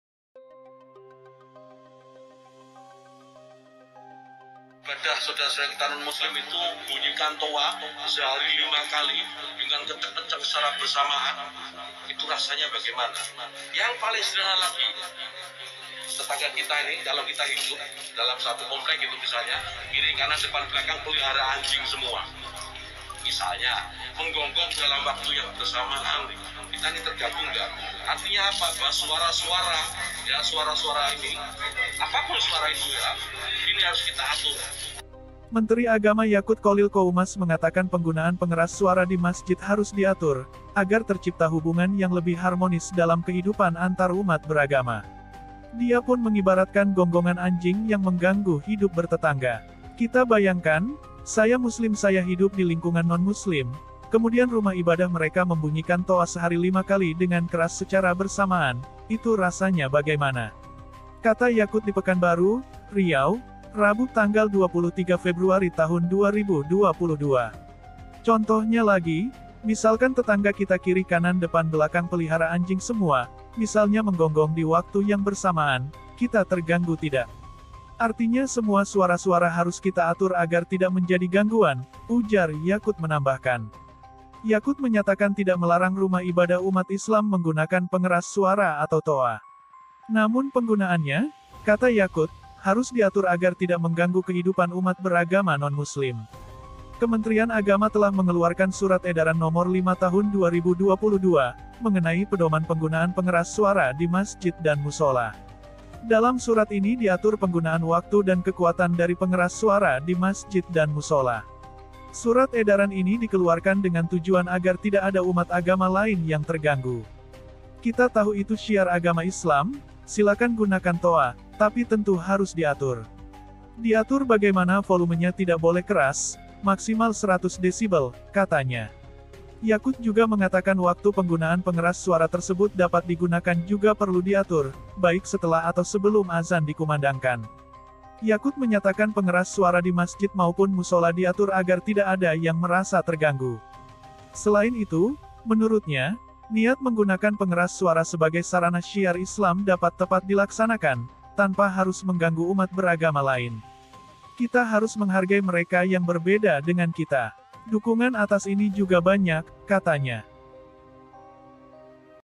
BIJAKLAH !! Menag Yaqut Cholil bandingkan aturan pengeras suara Masjid dengan gonggongan Anjing
Menteri Agama Yaqut Cholil Qoumas mengatakan penggunaan pengeras suara di masjid harus diatur, agar tercipta hubungan yang lebih harmonis dalam kehidupan antarumat beragama. Dia pun mengibaratkan gonggongan anjing yang menggangu hidup bertetangga.

"Kita bayangkan, saya Muslim saya hidup di lingkungan nonmuslim, kemudian rumah ibadah mereka membunyikan toa sehari lima kali dengan keras secara bersamaan, itu rasanya bagaimana?" kata Yaqut di Pekanbaru, Riau, Rabu (23/2/2022).

"Contohnya lagi, misalkan tetangga kita kiri kanan depan belakang pelihara anjing semua, misalnya menggonggong di waktu yang bersamaan, kita terganggu tidak? Artinya semua suara-suara harus kita atur agar tidak menjadi gangguan," ujar Yaqut menambahkan.

Yaqut menyatakan tidak melarang rumah ibadah umat Islam menggunakan pengeras suara atau toa. Namun penggunaannya, kata Yaqut, harus diatur agar tidak mengganggu kehidupan umat beragama nonmuslim...